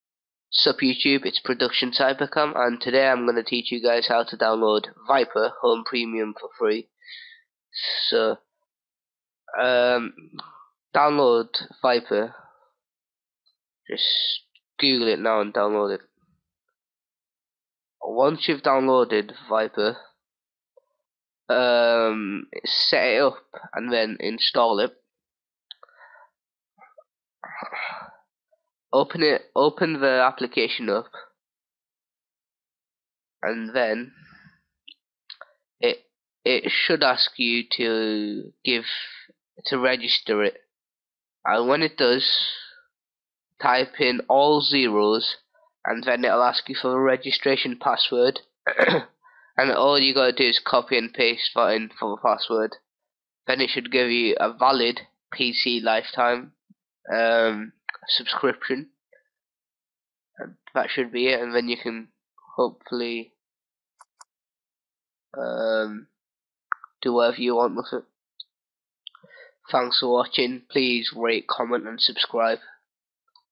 How to Register Vipre Antivirus for Free (UPDATED)
New download, you will need microsoft visual C++ (most people have this preinstalled, if not then google it)
then follow the instructions in the folder.

Thank's for watching, please check my channel and if you have any requests for tutorials please message me your idea's.